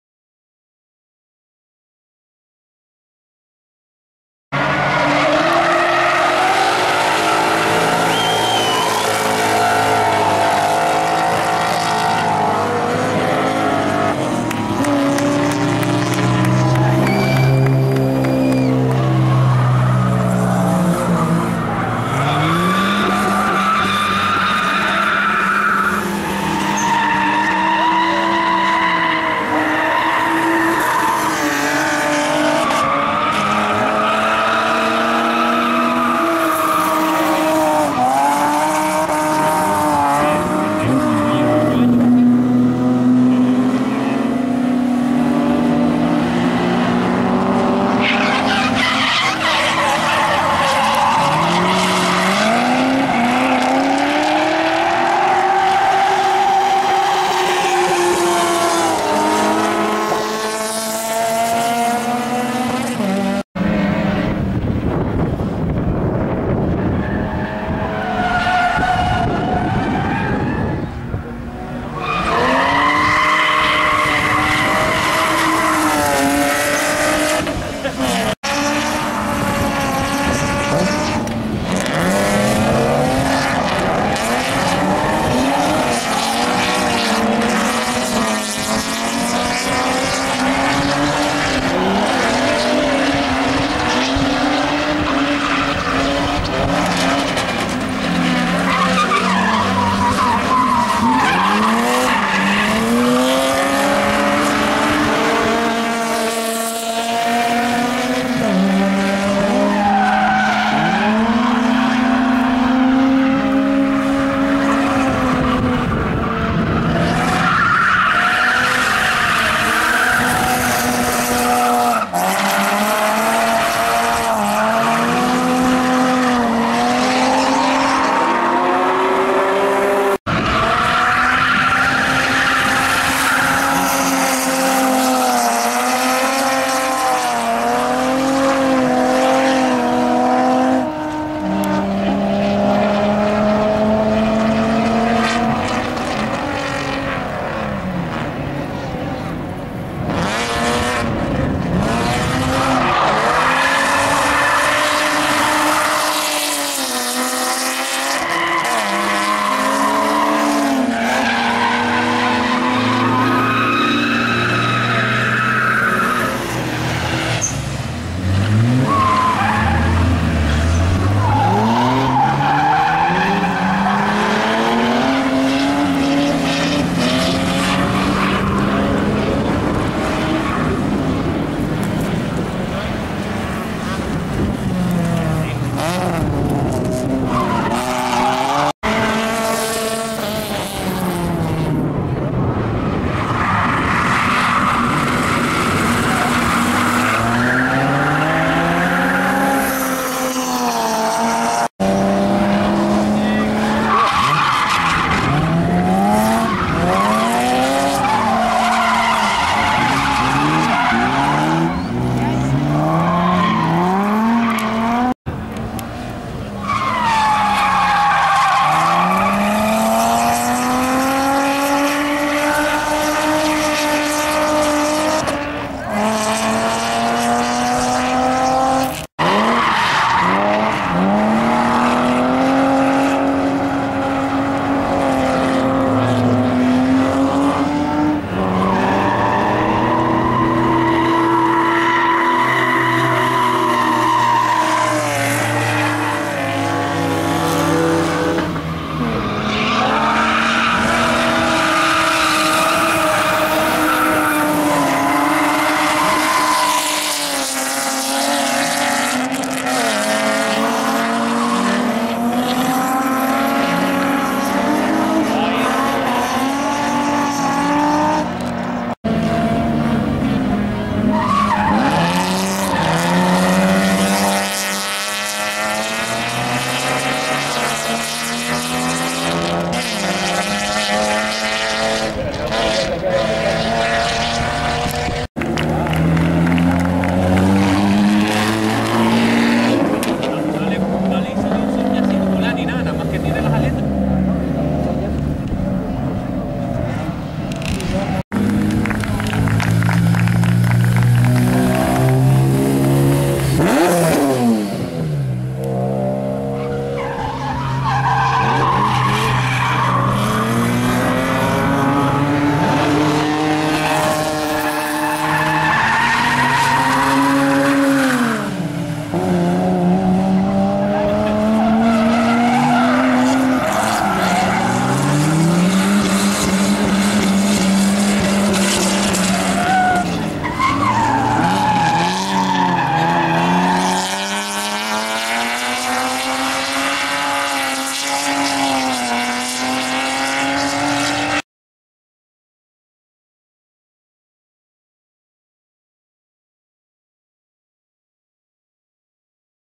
DRIFTING MASPALOMAS 19 FEB 2017 4º CHICHARREROS, CANARIONES, SUECOS HAY DE TOOOOO EN LOS COCHES
He intentado que salieran todos los que participaron, espero les gusten estos vídeos de track day en el circuito de Tarajalillo, un poco de drifting es bueno pa´l cuerpo. Un saludo a todos/as